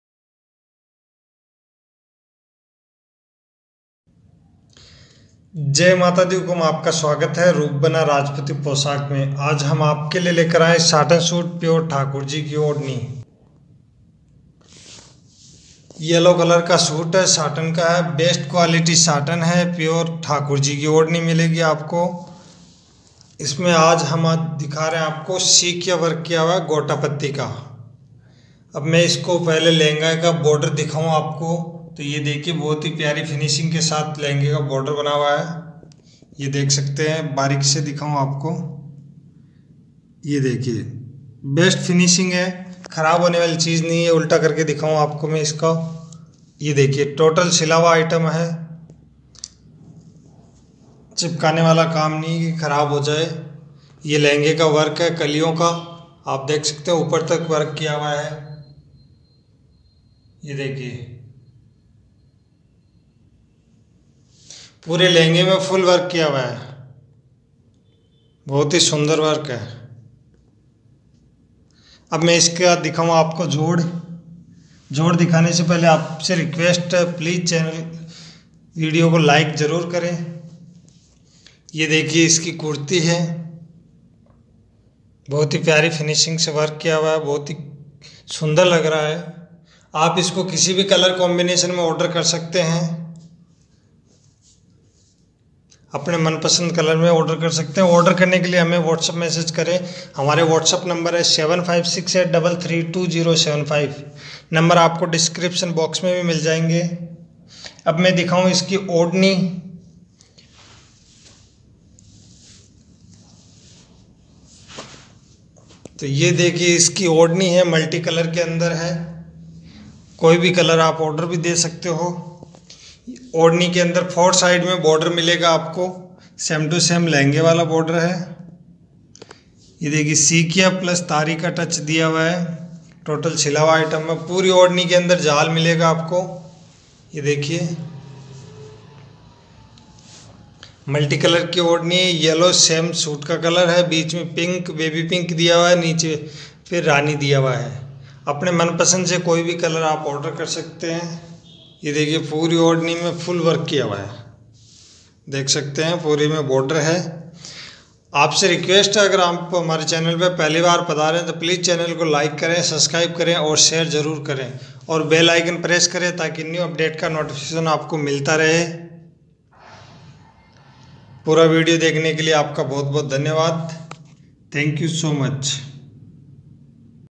गोटा पत्ती वर्क राजपूती पोशाक होलसेल प्राइस
हम आपके लिए लेकर आए हैं साटन का सूट प्योर की ओढ़नी
गोटा पत्ती सिख्या वर्क
बेस्ट क्वालिटी मैट्रियल
आर्डर करने के लिए हमें व्हाट्सएप मैसेज करें
साथ ही बेल आइकन प्रेस करें ताकि न्यू अपडेट का नोटिफिकेशन आपको मिलता रहे
पूरा वीडियो देखने के लिए आपका बहुत-बहुत धन्यवाद